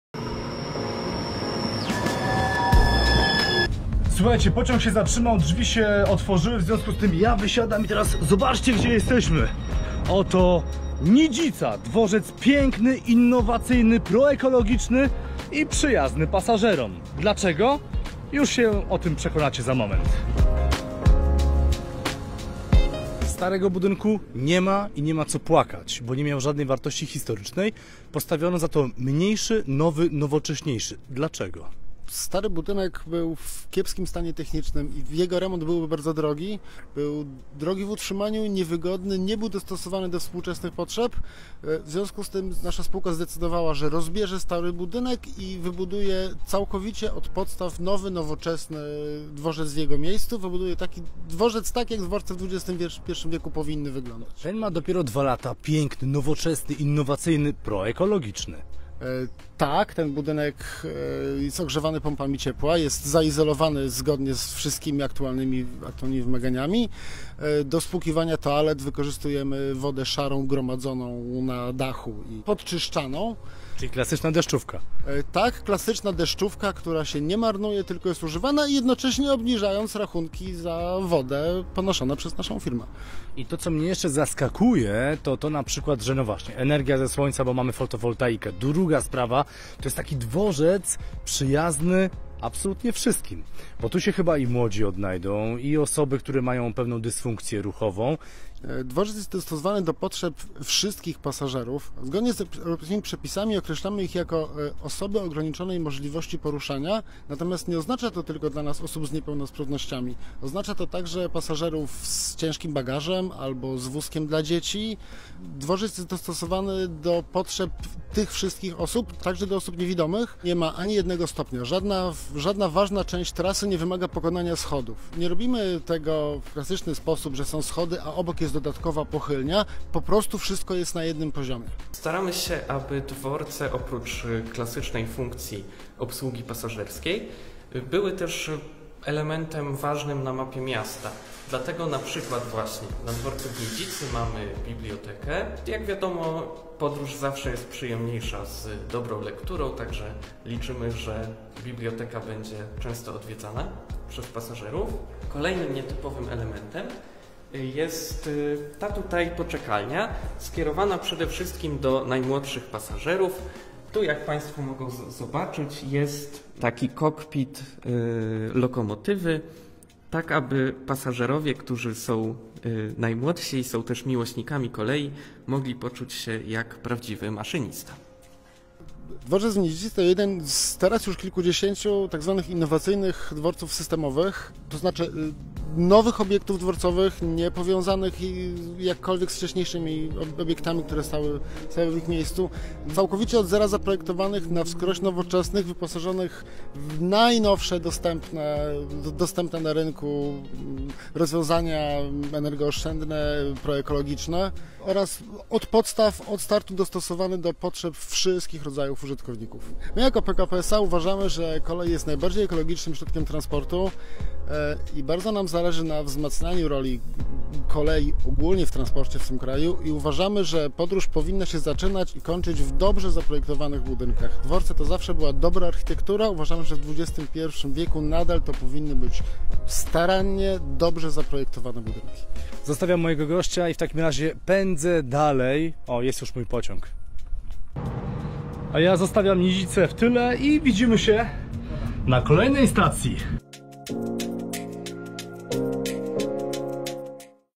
Poznaj Innowacyjny Dworzec Systemowy 𝗣𝗞𝗣 Nidzica! 🚉
🚅 Wakacje to czas podróży❗ Zapraszamy do obejrzenia kolejnego z serii filmu o dworcach 𝗣𝗞𝗣 S.A., który powstał we współpracy z @RadiowaJedynkaPR . 📻 🚉 👉 Innowacyjny Dworzec Systemowy Nidzica jest nowoczesny, proekologiczny oraz stworzony według autorskiego projektu Polskich Kolei Państwowych ❗ 🎥